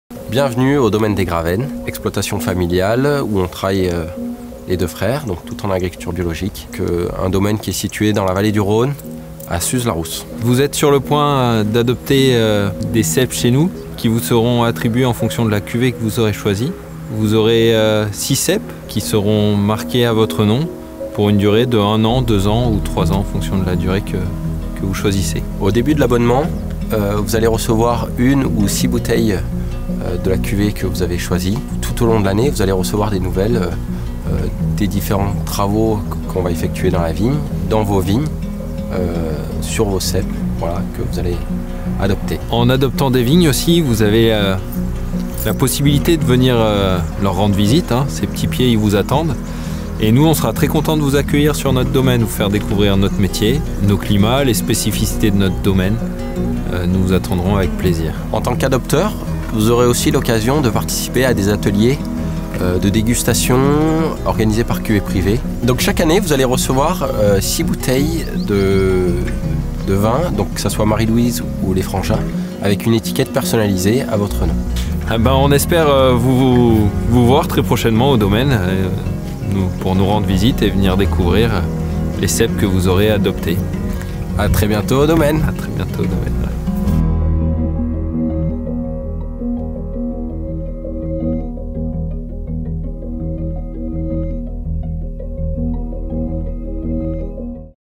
Luc et Rémi, les vignerons du Domaine des Gravennes
Une brève introduction du Domaine des Gravennes par Luc et Rémi, ainsi que des avantages à adopter des vignes chez Cuvée Privée